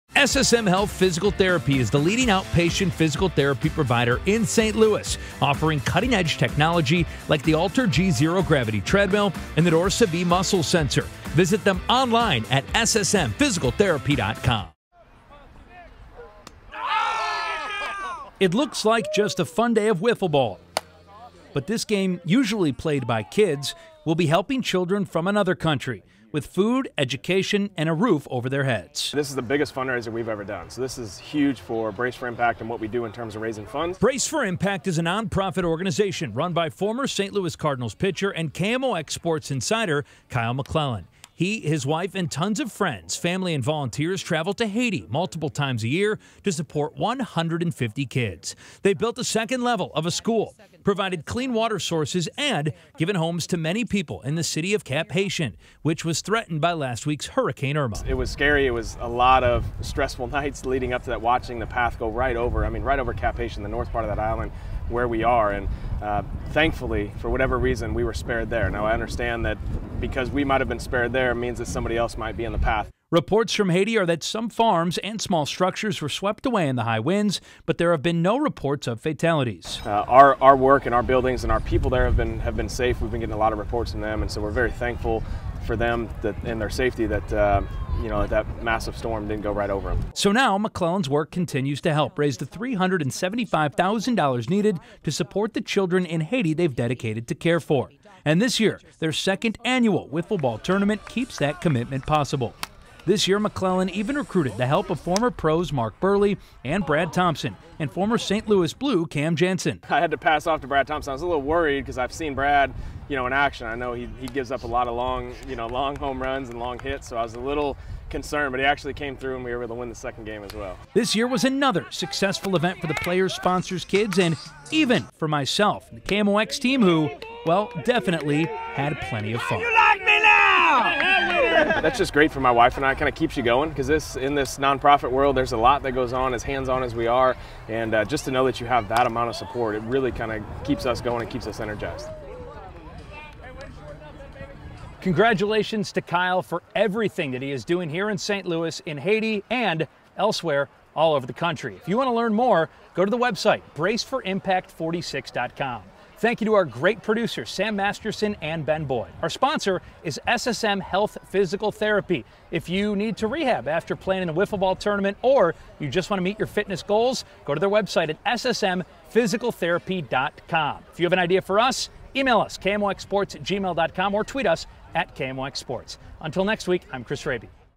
How Kyle McClellan Uses Wiffle Ball to Save Lives
Our sports insider travels multiple times a year to a city in the northern tip of Haiti to improves the lives of local children.